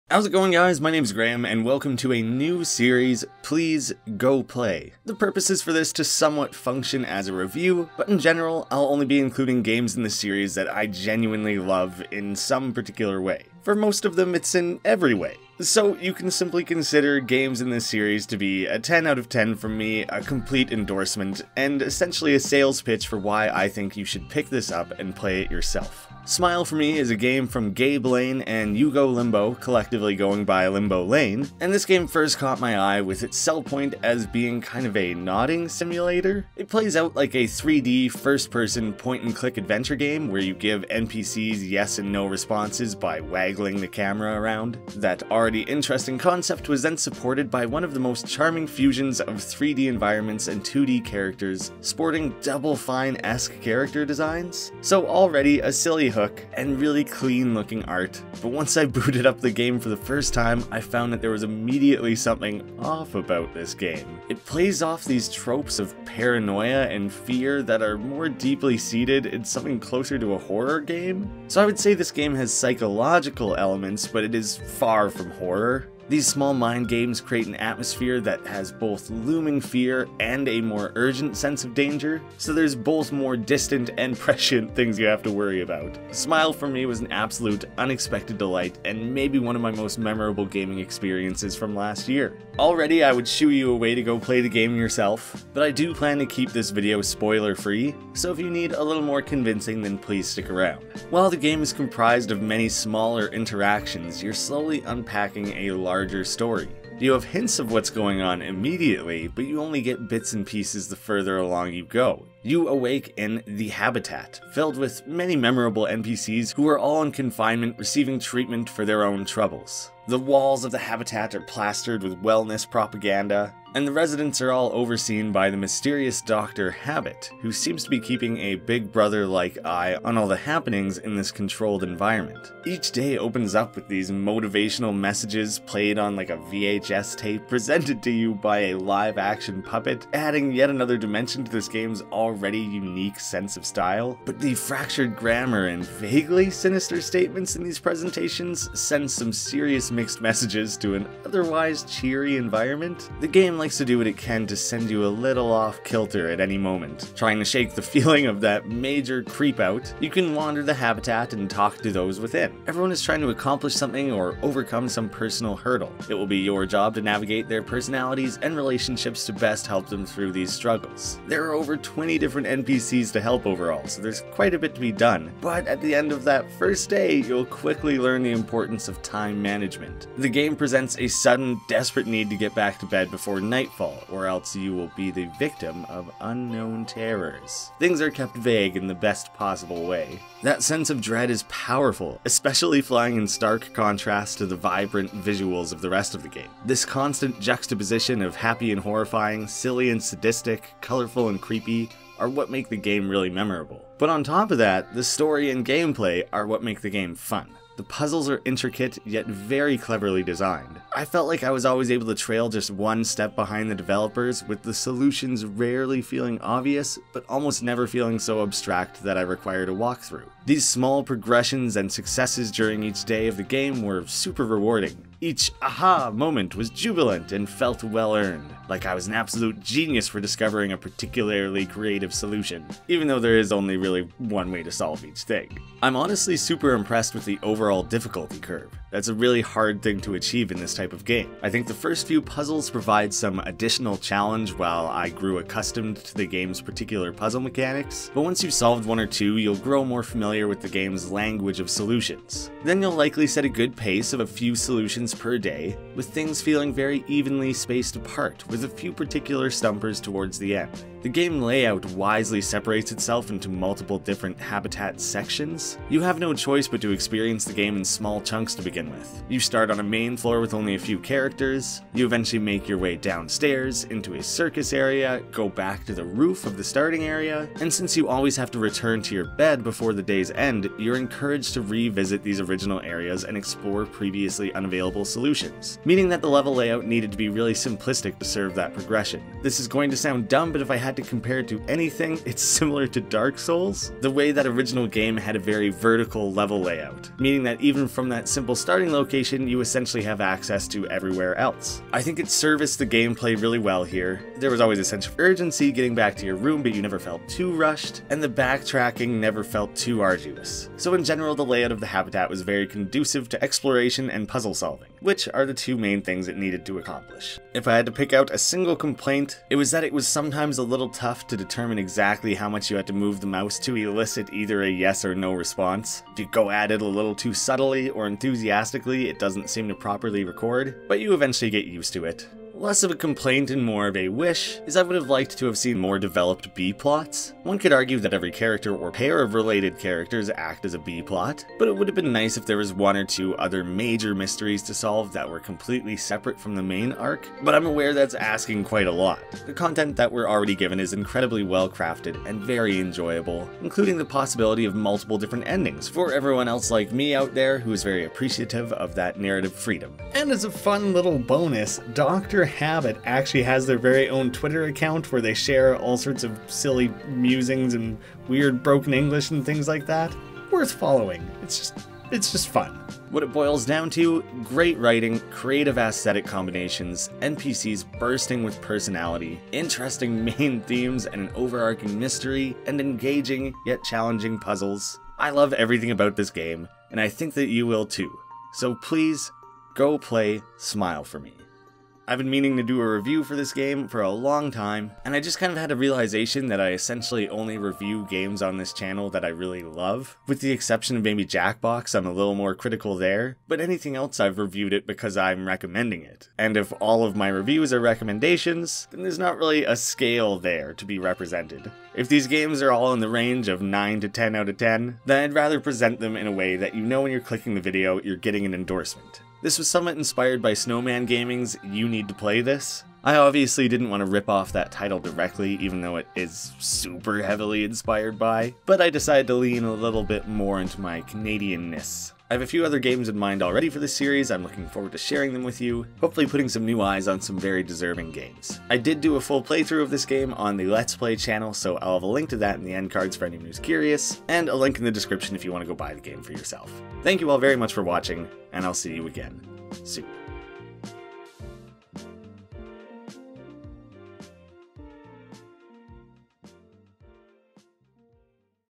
Please, go play... Smile For Me
In Smile For Me, you have joined a community for unhappy people: The Habitat. Your job is to interact with the whacky individuals and do what you can to improve their moods. Just one thing. DO NOT stay up past curfew...

Smile For Me Steam description:
"A heartbroken clown, wayward souls, and juicy mechanical lips. As “The Big Event" draws near, help the troubled residents of the Habitat and de-mask the mysterious Doctor Habit... before it's too late."

Smile For Me completely caught me off guard! I thought I was playing a jovial game, with a gimmicky gameplay mechanic. Turns out this game has one of the most immediately compelling tones I've ever experienced. My Smile For Me review is nothing but positive, and I cannot recommend it enough!

So Please, Go Play Smile For Me!